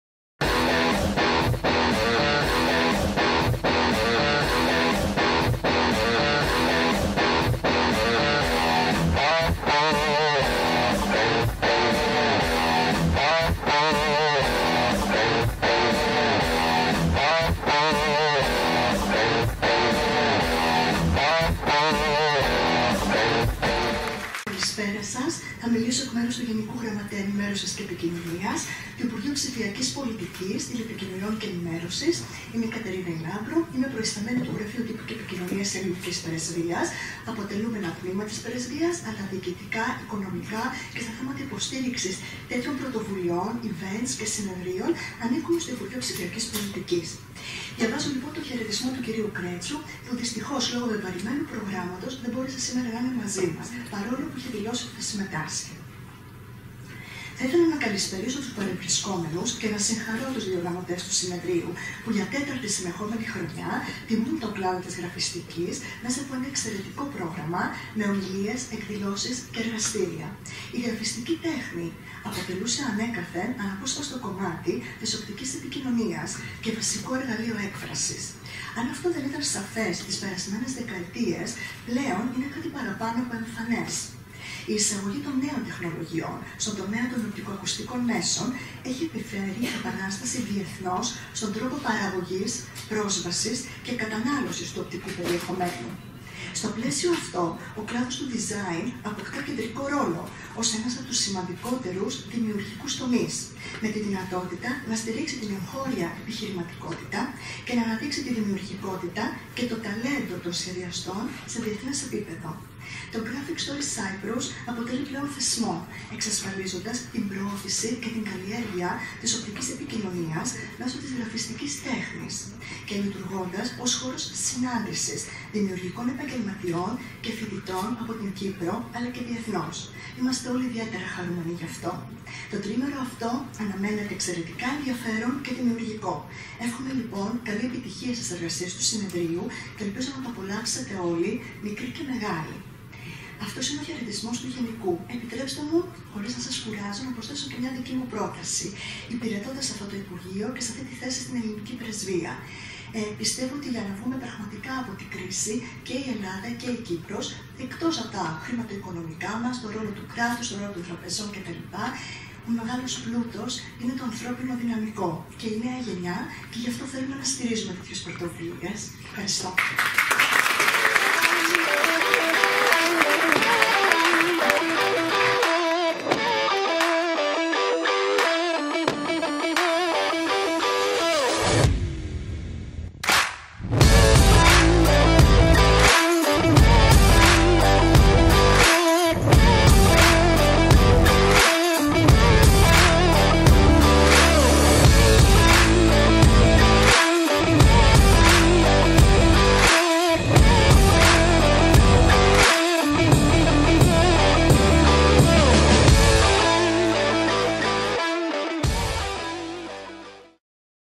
H ΓΓΕΕ στο Graphic Stories Cyprus 2018
Χαιρετισμός από την Δρ Κατερίνα Λάμπρου, Προϊσταμένη Γρ. Τύπου και Επικοινωνίας Πρεσβείας της Ελλάδας στην Κύπρο εκ μέρους του Γενικoύ Γραμματέα Ενημέρωσης και Επικοινωνίας, Λευτέρη Κρέτσου στο 4ο Συνέδριο Σχεδιαστών Οπτικής Επικοινωνίας Κύπρου, Graphic Stories Cyprus 2018.

Το Συνέδριο πραγματοποιήθηκε με τη συνεργασία της Τράπεζας Κύπρου και του Πολιτιστικού Ιδρύματος της Τράπεζας Κύπρου και τελεί υπό την αιγίδα των Πολιτιστικών Υπηρεσιών του Υπουργείου Παιδείας και Πολιτισμού της Κυπριακής Δημοκρατίας, του Υπουργείου Ψηφιακής Πολιτικής, Τηλεπικοινωνιών και Ενημέρωσης της Ελληνικής Δημοκρατίας, καθώς και του Δήμου Λευκωσίας.

Επίσημοι χορηγοί: Parachute Type Foundry, Σχολή Καλών κι Εφαρμοσμένων Τεχνών Αιγαία και Kemanes Printshop.

Χορηγοί Επικοινωνίας: εφημερίδα «Πολίτης» - «Παράθυρο», «Πολίτης 107.6», ΡΙΚ.